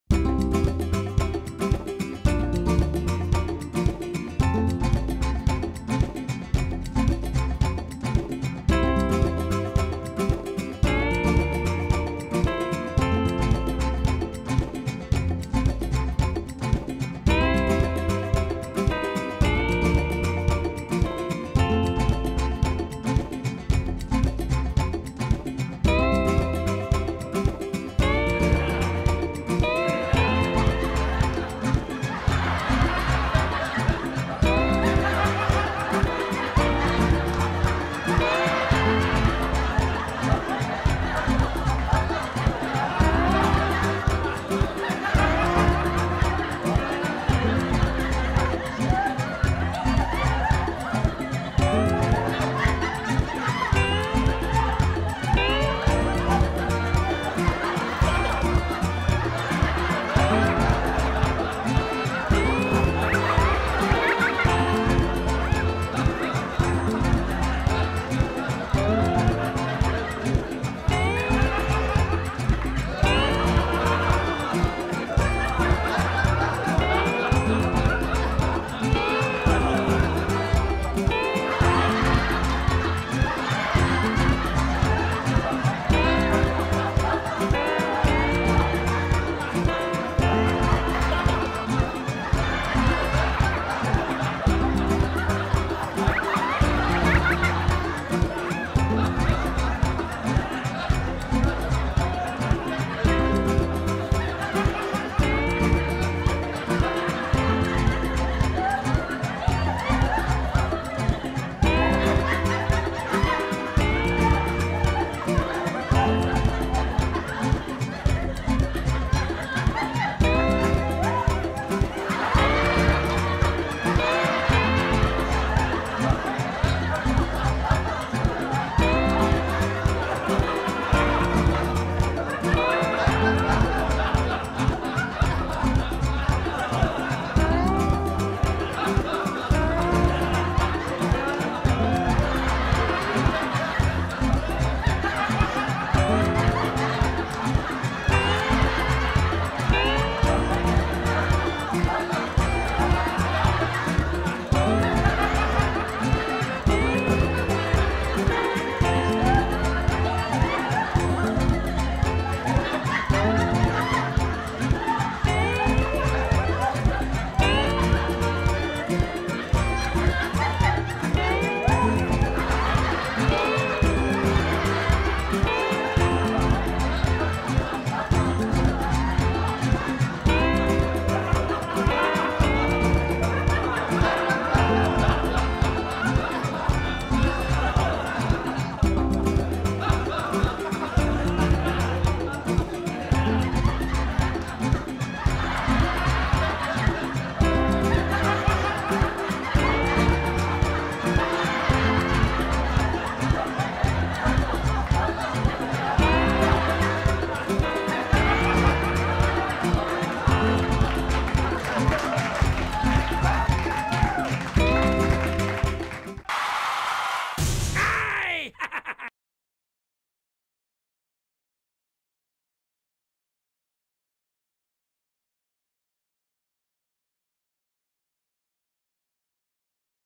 A passenger with long sugar cane PRANK
A passenger trie to boar a bus with long sugar cane.

FUNNY!